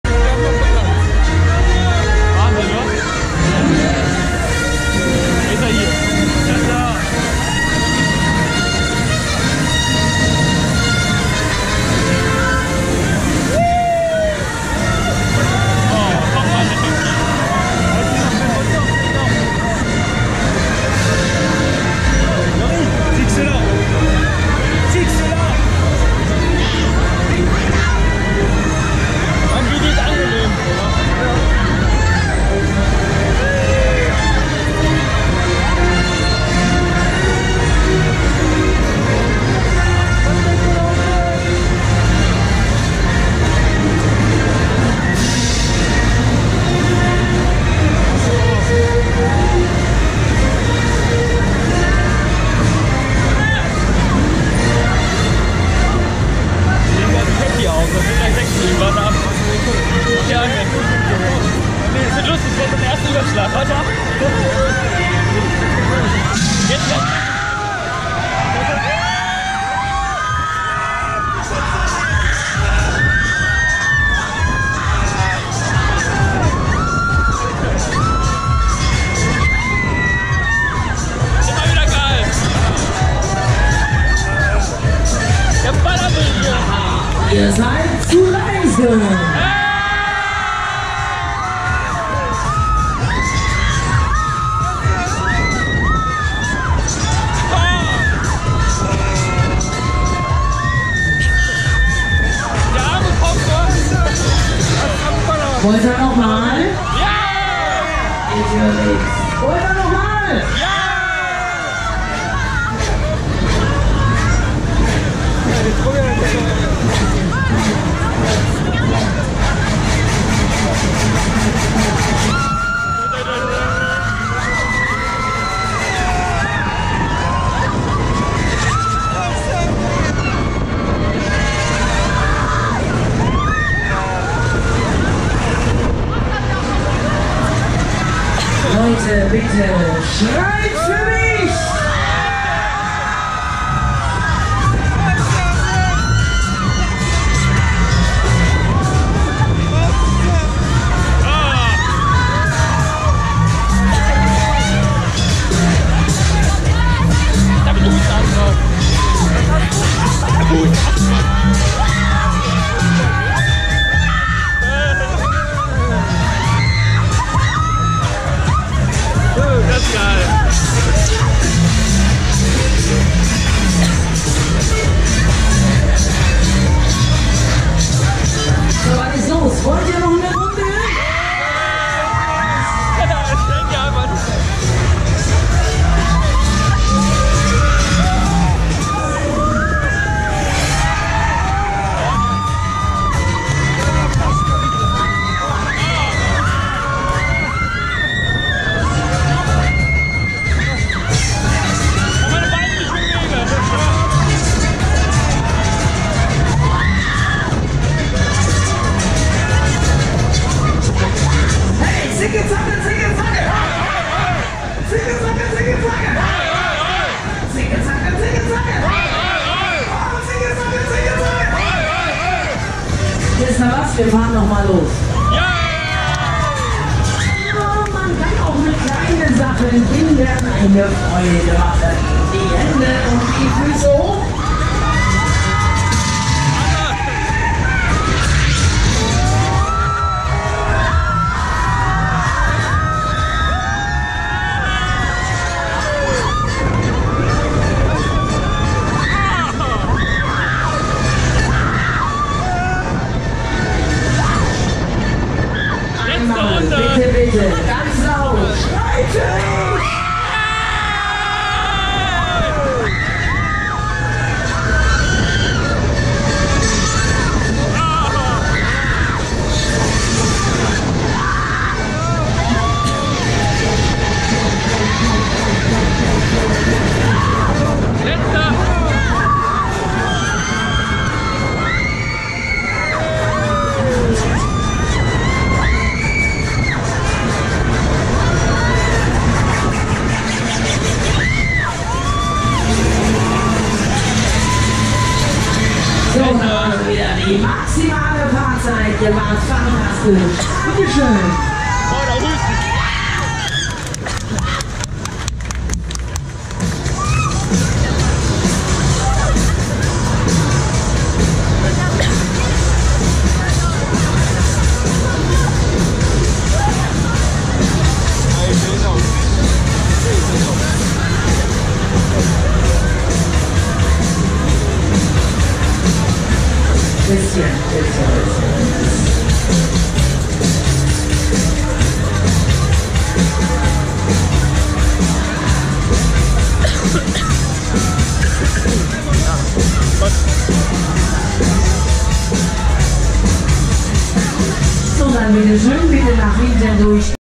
Transformer - Schmidt (Onride) Schueberfouer Luxemburg 2019
Fahrpreis: 5,00€

Ein Onride-Video vom "Transformer" von Schmidt, gefilmt auf der Schueberfouer Luxemburg 2019.